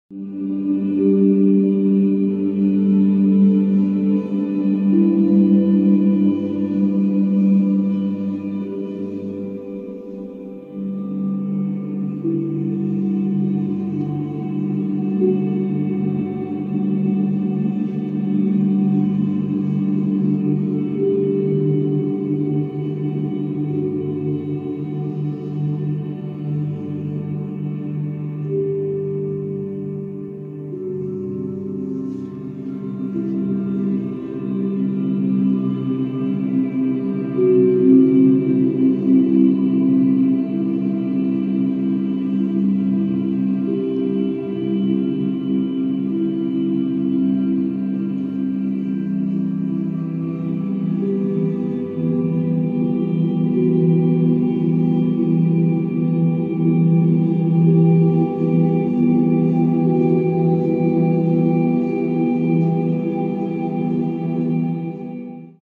Sound Bath Key of G Drone
Soundwell Boise plays its signature G-drone piece on crystal singing bowl (16-inch diameter G-note bowl), electric keyboard, electric guitar, and steel tongue drum. Musicians pictures are Evelyn Hadden (keys), Kara Gillette (drum), Gordon Rosenberg (bowl), and Christian Gillette (guitar). Our fifth member, Debby Rosenberg, is holding the camera for this track.